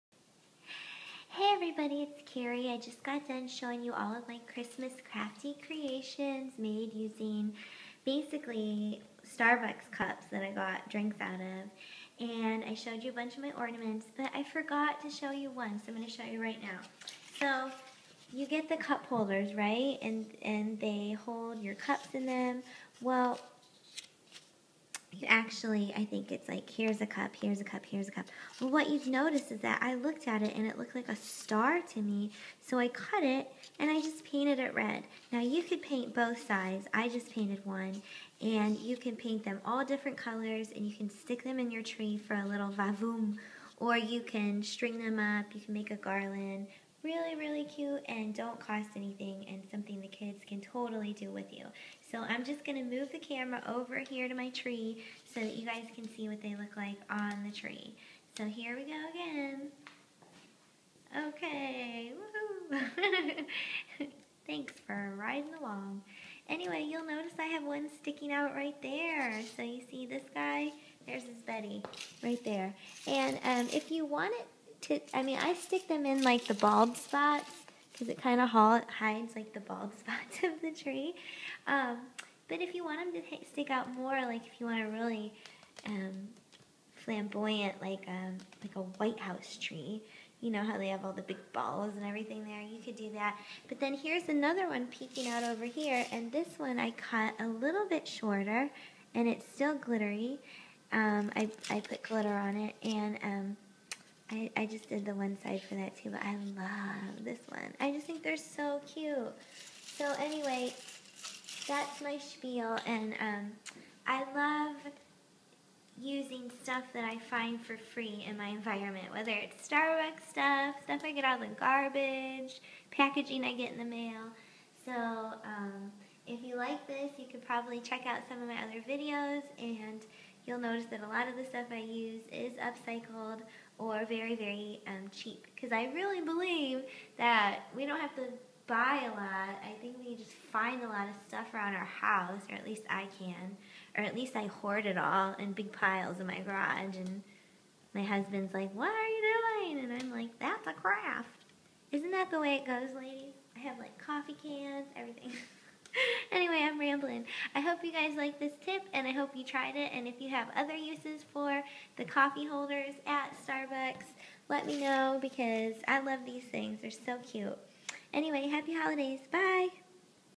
❤️DIY CRAFT! Starbucks Christmas Ornaments!❤️
Love using the coffee-holders from Starbucks to make giant stars for the Christmas tree.
Xoxo
Kerry

PS
What kind of crafty things do you make with them? I love seeing how creative ya'll are!

Happy Holidays!

Don't forget to say hi on Instagram: @kerryluvcat (I follow back!)

Hope Everyone has an amazing New Year!!! Xoxo